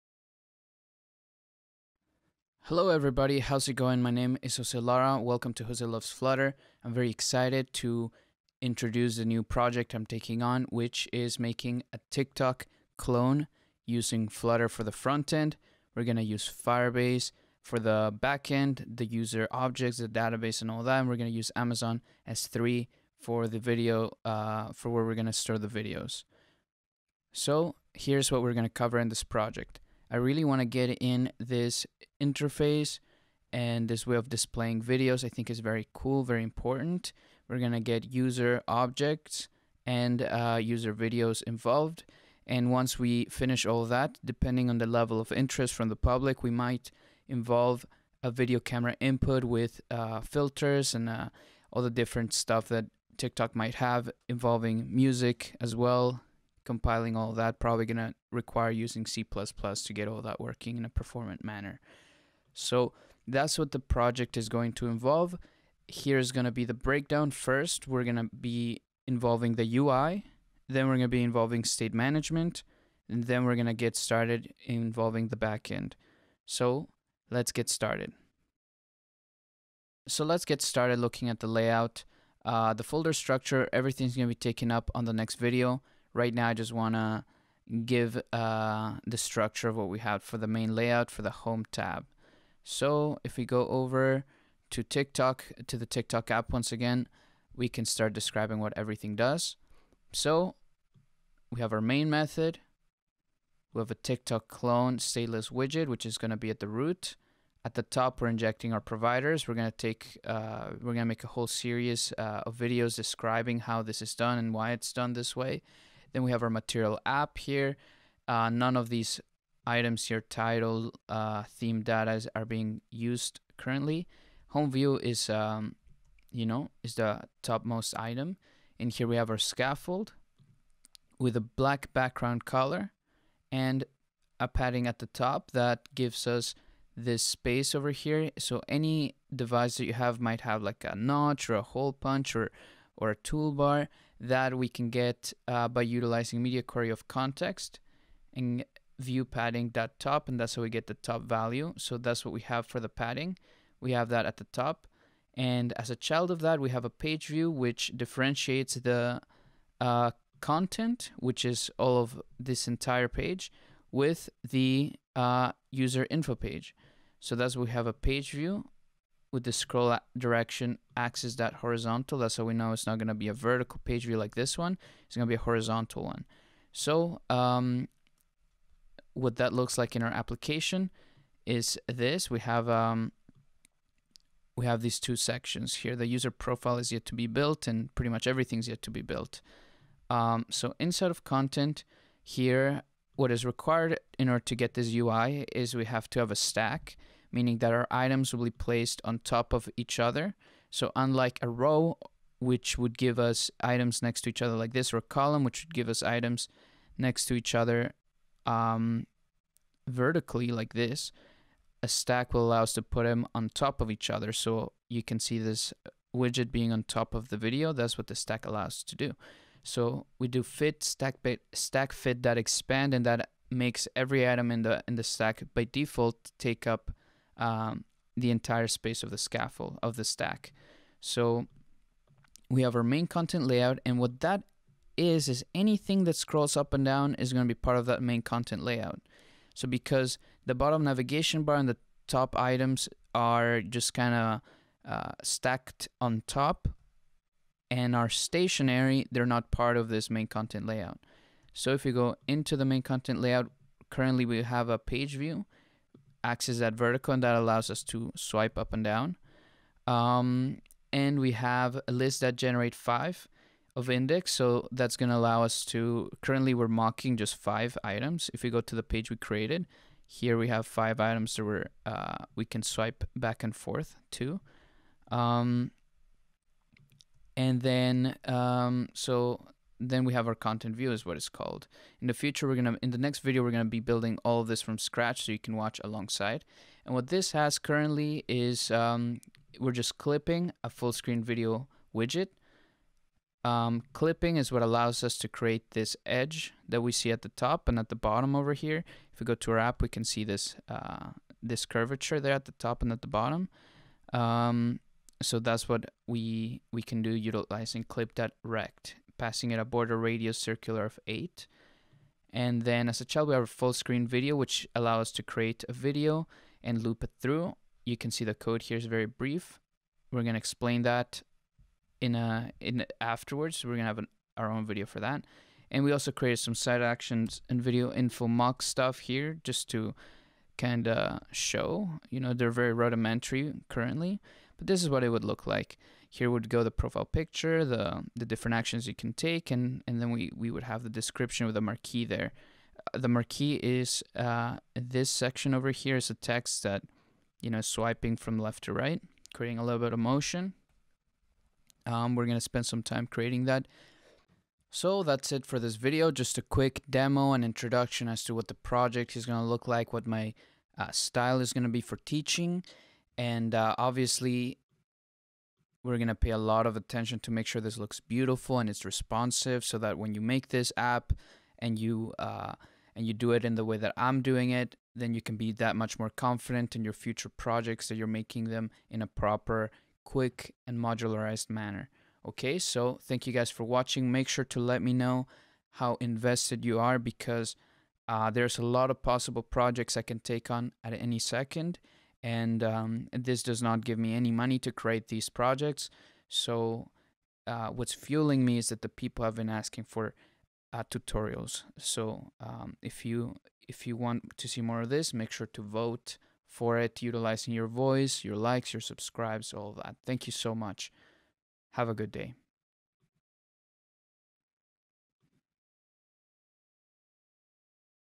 2020 Complete Flutter Full Tik Tok Clone Part 1
This video is an introduction which shows you how to create a Flutter Tik Tok Clone, we are going to be covering the user, and video flow.